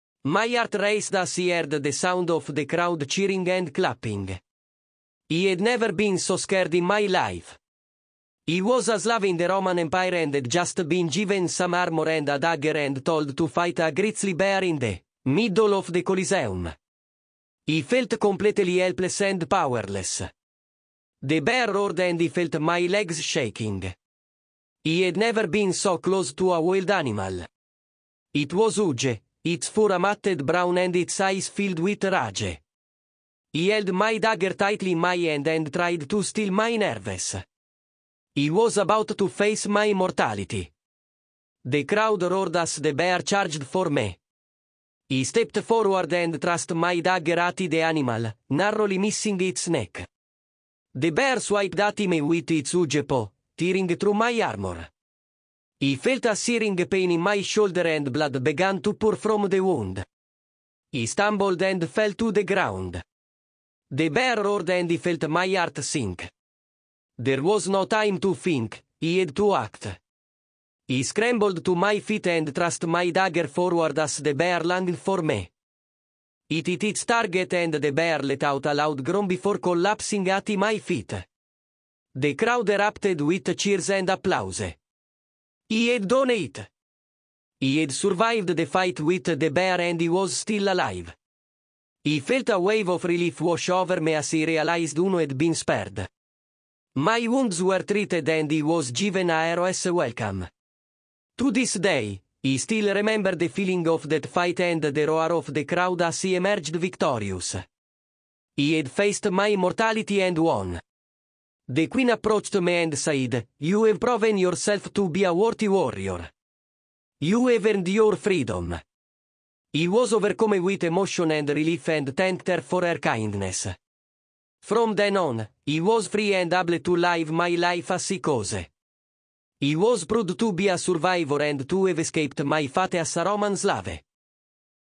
First Person Experience of Roman Gladiator
My heart raced as I heard the sound of the crowd cheering and clapping. I had never been so scared in my life. I was a slave in the Roman Empire and had just been given some armor and a dagger and told to fight a grizzly bear in the middle of the coliseum. I felt completely helpless and powerless.

The bear roared and I felt my legs shaking. I had never been so close to a wild animal. It was huge, its fur a matted brown and its eyes filled with rage. I held my dagger tightly in my hand and tried to steel my nerves. I was about to face my mortality.

The crowd roared as the bear charged for me. I stepped forward and thrust my dagger at the animal, narrowly missing its neck. The bear swiped at me with its huge paw, tearing through my armor. I felt a searing pain in my shoulder and blood began to pour from the wound. I stumbled and fell to the ground.

The bear roared and I felt my heart sink. There was no time to think, I had to act. I scrambled to my feet and thrust my dagger forward as the bear lunged for me. It hit its target and the bear let out a loud groan before collapsing at my feet.

The crowd erupted with cheers and applause. I had done it! I had survived the fight with the bear and I was still alive. I felt a wave of relief wash over me as I realized I had been spared.

My wounds were treated and I was given a hero's welcome. To this day, I still remember the feeling of that fight and the roar of the crowd as I emerged victorious. I had faced my mortality and won.

the queen approached me and said, “You have proven yourself to be a worthy warrior. You have earned your freedom.” I was overcome with emotion and relief and thanked her for her kindness.

From then on, I was free and able to live my life as I chose. I was proud to be a survivor and to have escaped my fate as a Roman slave.